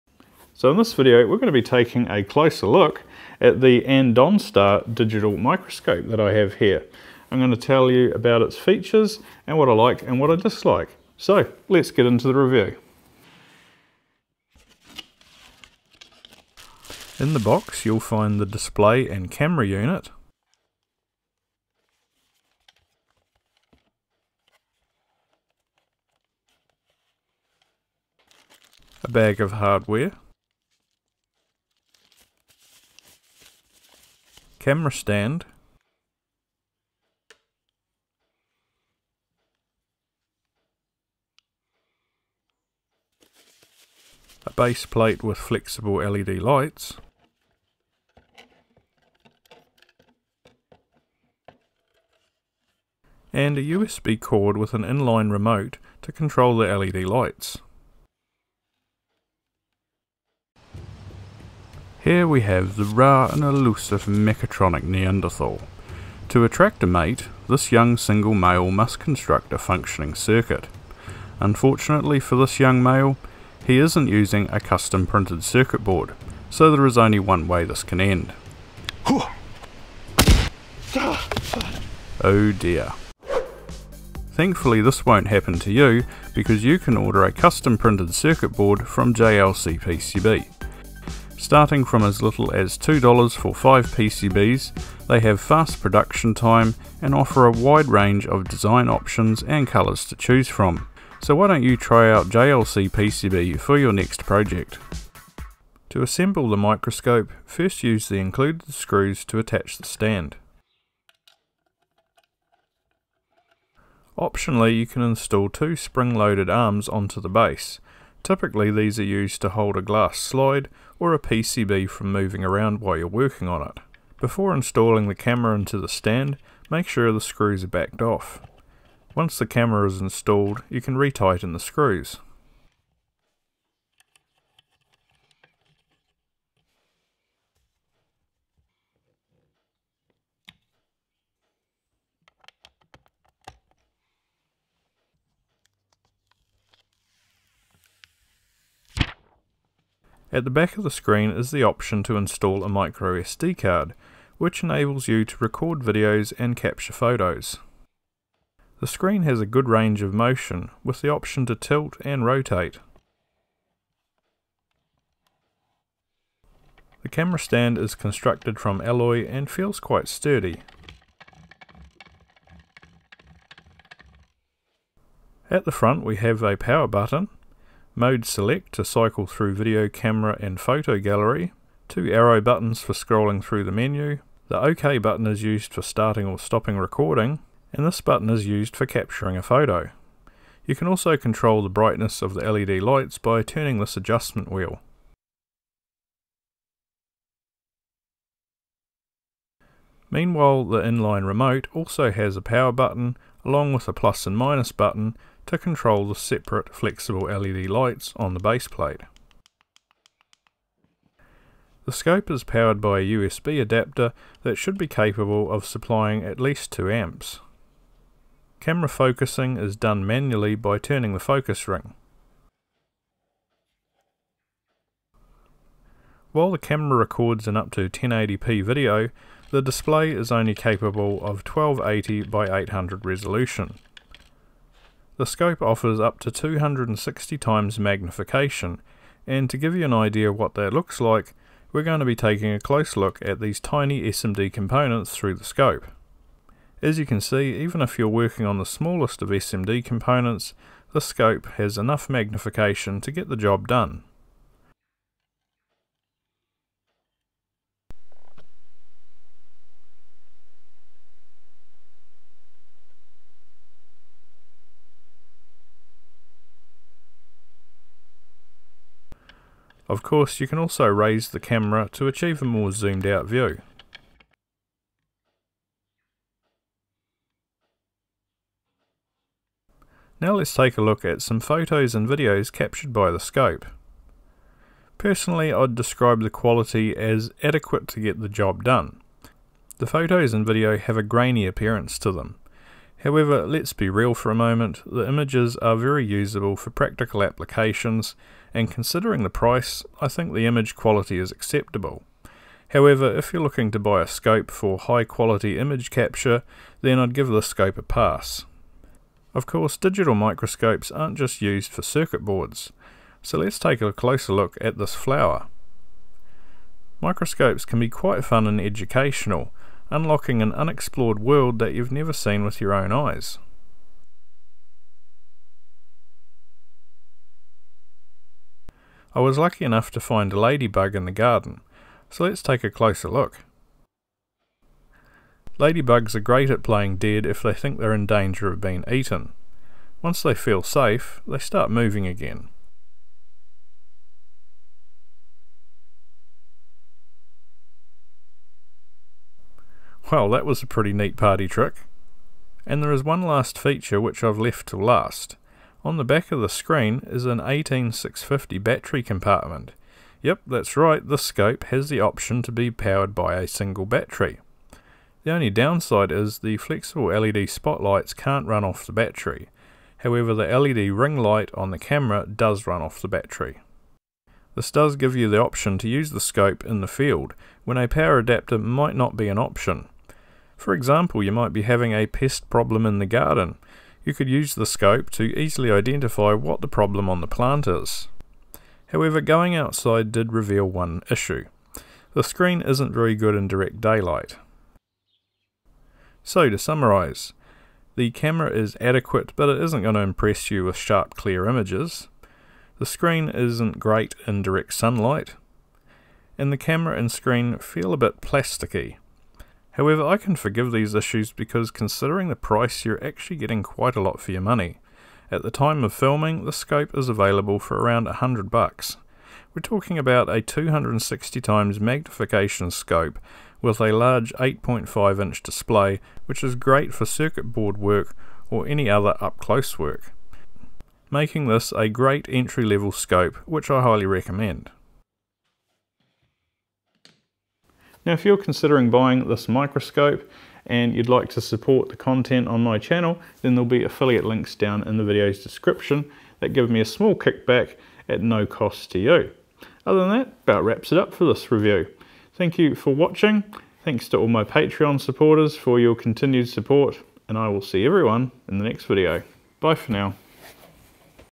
This is a Bargain! II Andonstar AD208 Digital Microscope 8.5" LCD Review
5Pcs from as little as $2

0:00 intro
0:14 unboxing
1:00 Ad
1:46 assembly
2:30 features
3:45 using the scope
6:36 summary